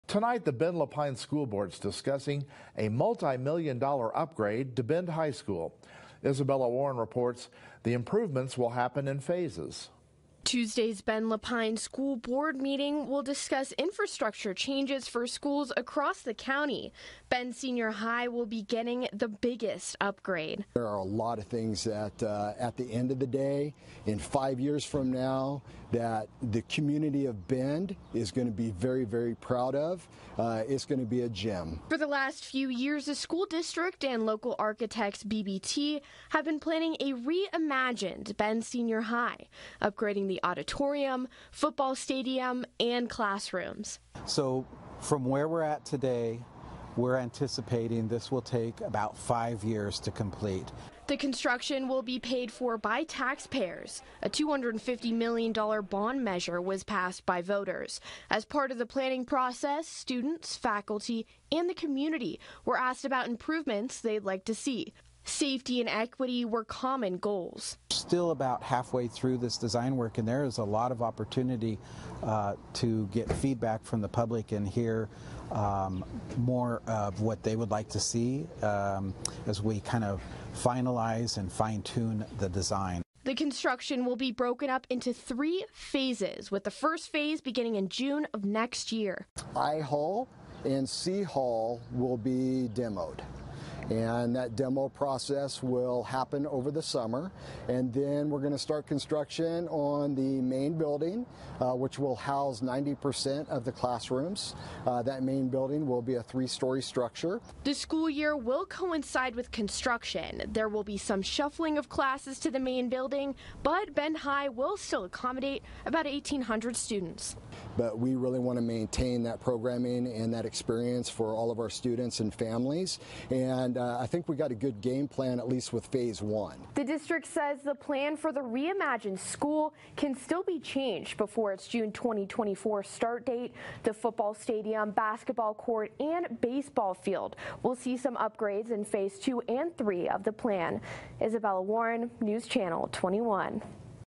'It's going to be a gem': details of new 'reimagined' Bend Senior H.S. shared before school ...
The Bend-La Pine School Board gets an update Tuesday evening on a variety of new construction and upgrades done over the summer at various schools. But a centerpiece presentation provides new details about the new, "reimagined" Bend Senior High School, advancing plans in the works for several years.